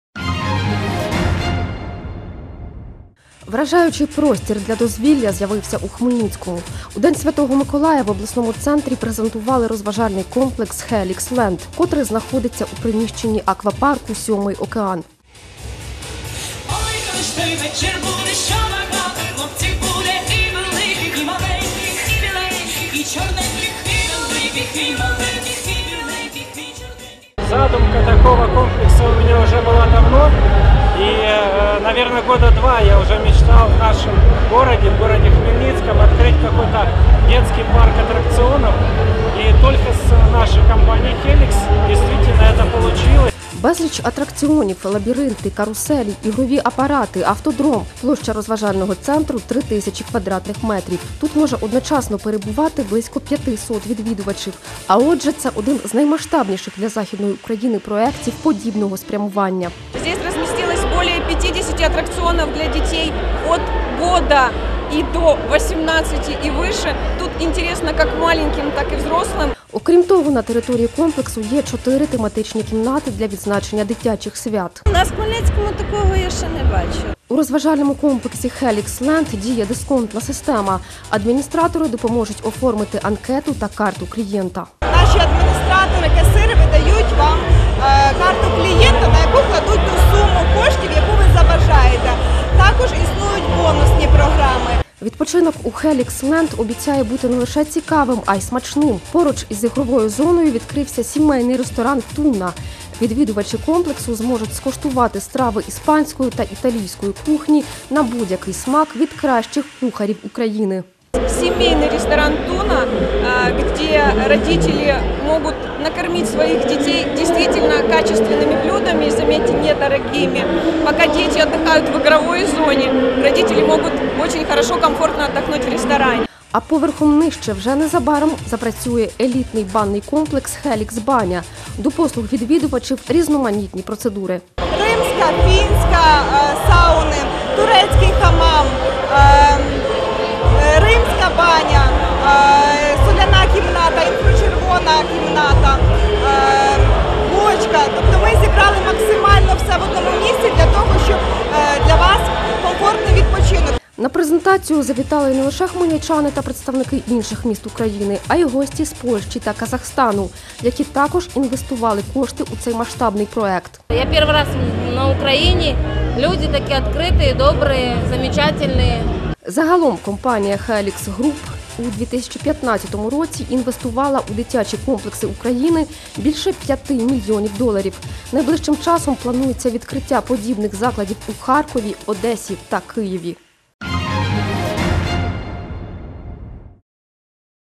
Спецрепортаж HELIX LAND 03 07
Відкриття HELIX LAND у Хмельницькому.